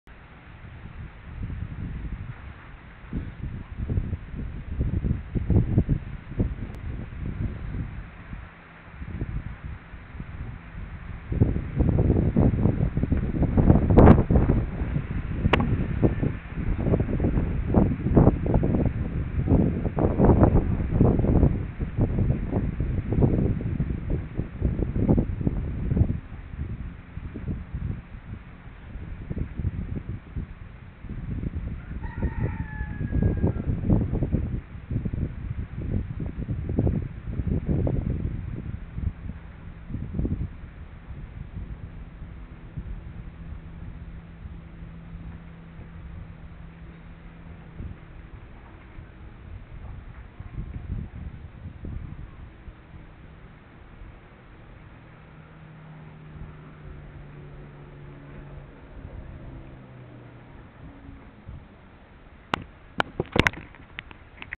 Maupiti Morning Rush Hour Traffic Near Collision
Given a few more years, and hundred or more attempts, the kayaker and motor boat could have potentially collided.